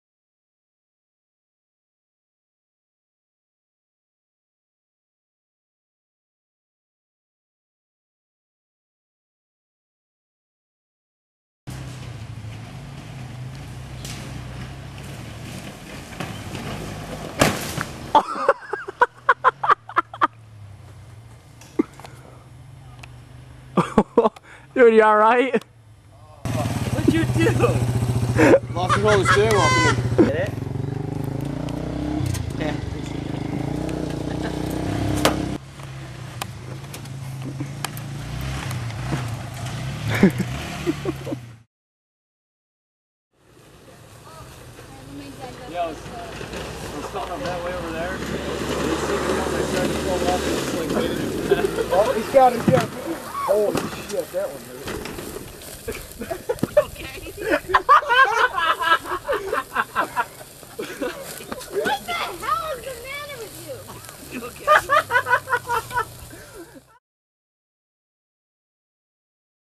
Golf cart hare scramble
Riding and racing a golf cart with a minor suspension lift through the woods.